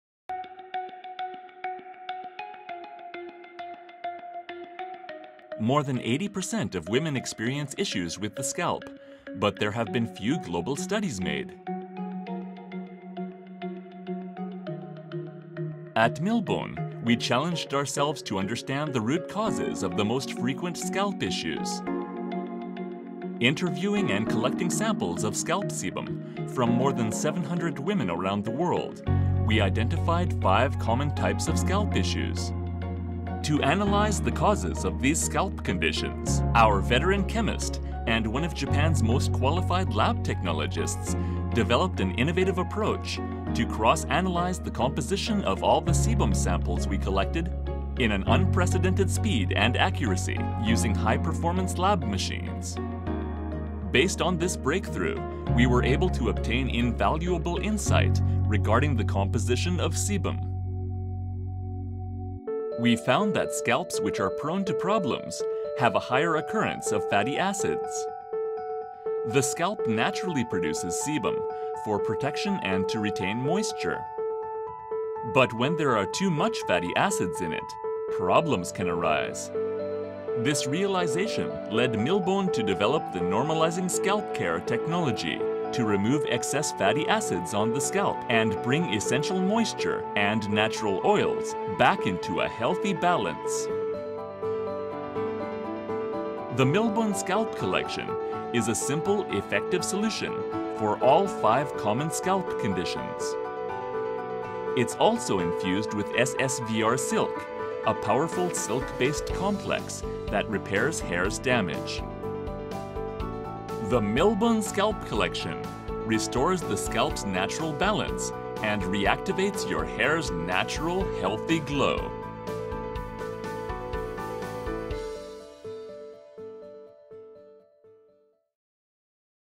Groundbreaking Scalp Research
Milbon's latest worldwide research exposed a single root cause for all 5 common scalp conditions (itchiness, dandruff, dryness, oiliness, and odor): excess fatty acids on the scalp.